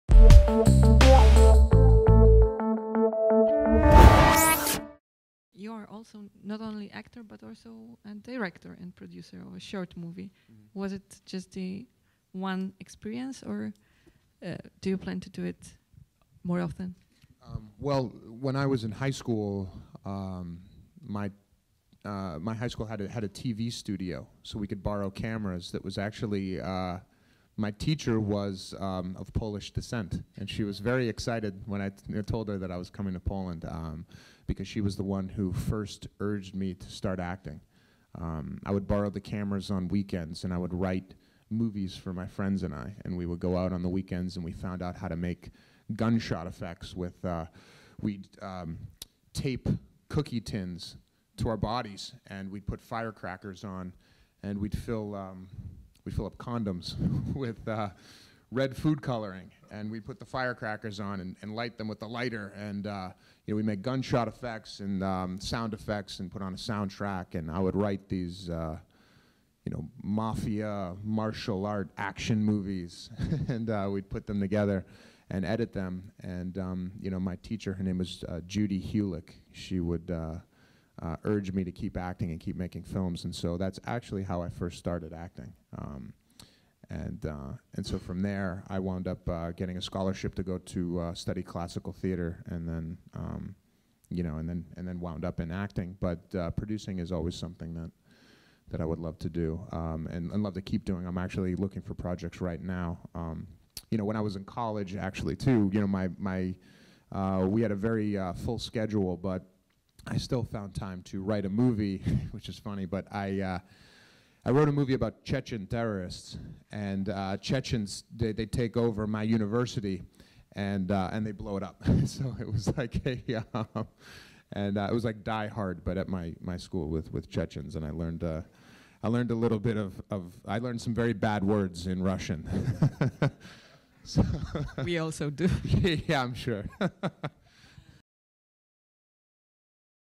Interview with Joe Manganiello (True Blood) part 4.
gazeta.tv presents: Interview with Joe Manganiello.
Wywiad z Joe Manganiello, aktorem grającym wilkołaka Alcide Herveaux w Serialu True Blood (Czysta Krew).